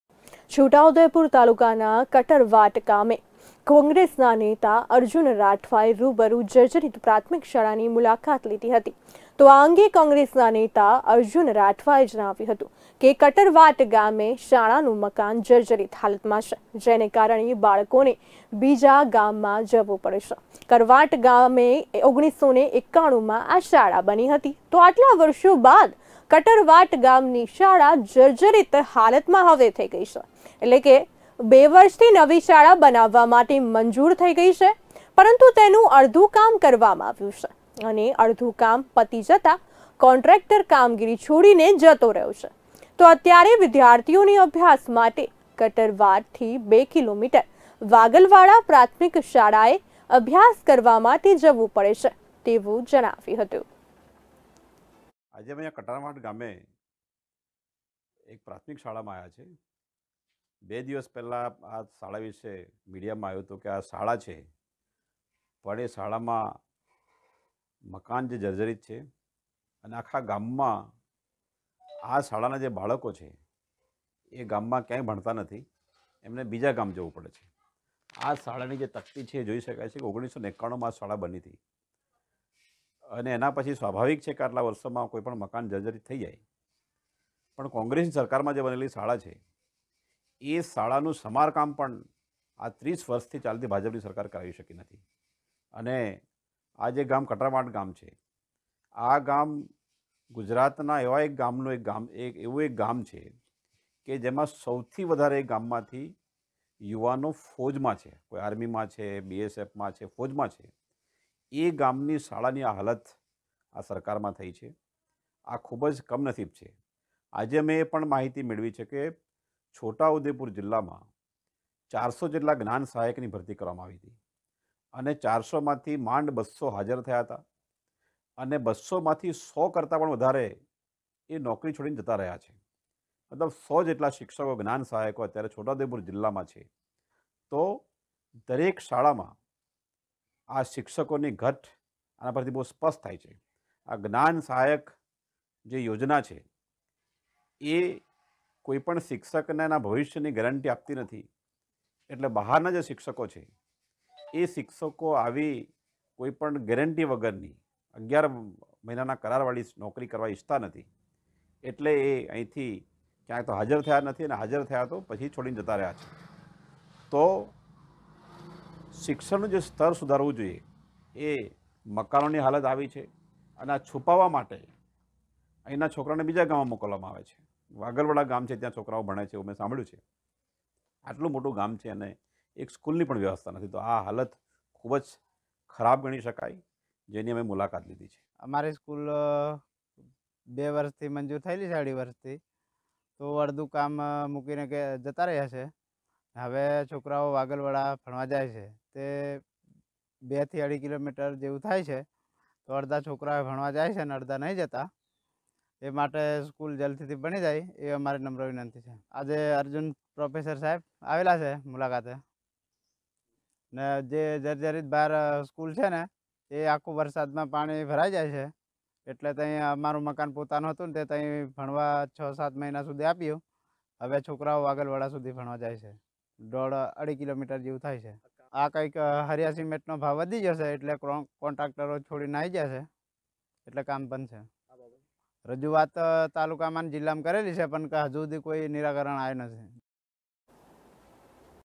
GUJARATI NEWS : કટરવાટ ગામે જર્જરીત શાળાની મુલાકાત કોંગ્રેસના નેતાએ કરી
છોટાઉદેપુર તાલુકાના કટરવાટ ગામે કોંગ્રેસના નેતા અર્જુન રાઠવાએ રૂબરૂ જર્જરીત પ્રાથમિક શાળાની મુલાકાત લીધી હતી.આ અંગે કોંગ્રેસના નેતા અર્જુન રાઠવાએ જણાવ્યું હતું કે કટરવાટ ગામે શાળાનું મકાન જર્જરીત છે જેના કારણે બાળકોને બીજા ગામમાં જવુ પડે છે.